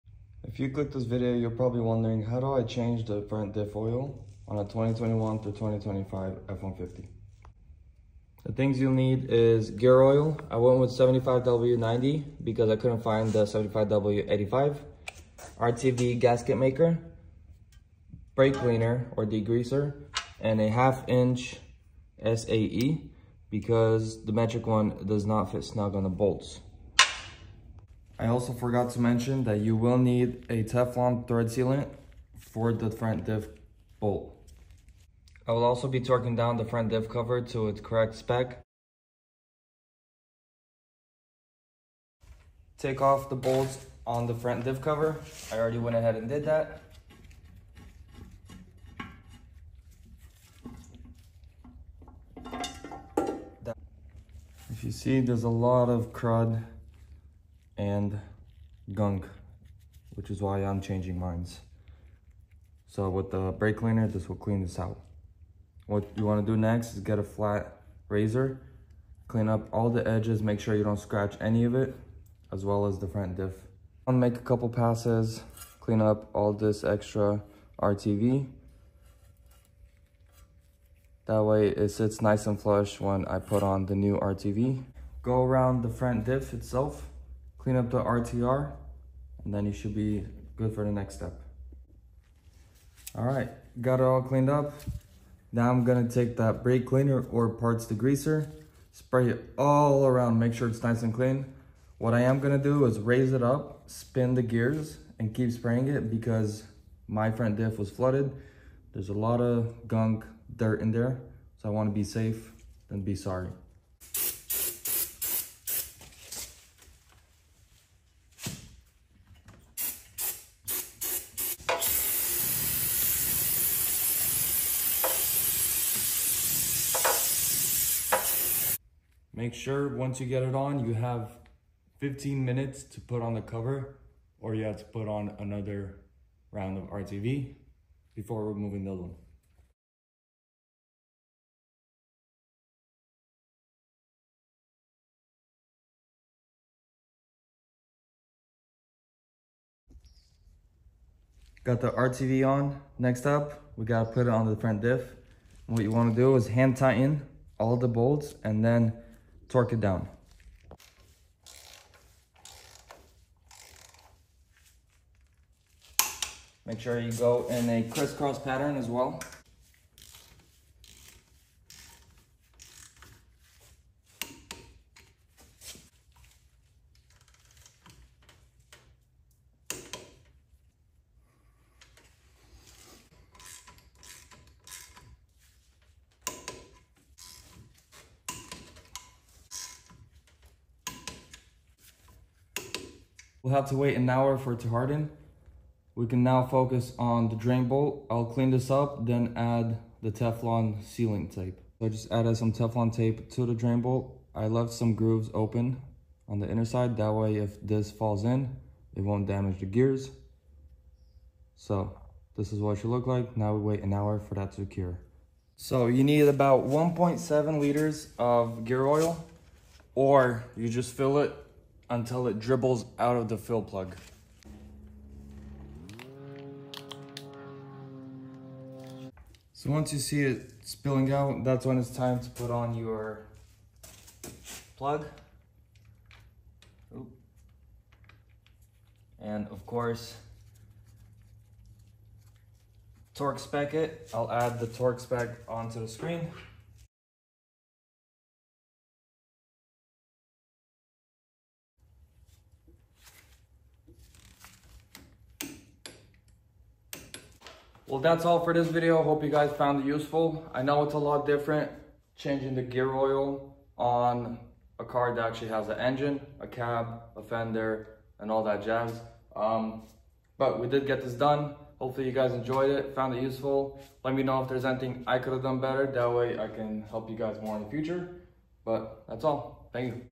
How to Change Front Differential Fluid on a 2021–2025 Ford F-150 (Step-by-Step)
In this video, I show you step-by-step how to change the front differential gear oil on a 2021–2025 Ford F-150. This guide works for most trims and engines — including XL, XLT, Lariat, King Ranch, Platinum, Tremor, and Limited — as long as your truck is 4WD.

You’ll learn how to:
 • Safely lift and support your truck
 • Locate and remove the front differential fill and drain plugs
 • Drain out the old gear oil completely
 • Refill with fresh SAE 75W-90 synthetic hypoid gear oil
 • Torque everything to spec and check for leaks

I’ll also cover how much fluid the front diff takes, what tools you need, and tips to make the job easier — including how to properly clean the magnet and refill without making a mess.

📦 Recommended Gear Oil:
 • Valvoline 75W-90
 • Capacity: ~1.7 L (1.8 qt) for most 4WD models

⚙️ Tools Needed:
 • Drain pan
 • Torque wrench
 • Safety stands & gloves

💡 Pro Tip: Always remove the fill plug first — that way you know it can be refilled before draining the old fluid!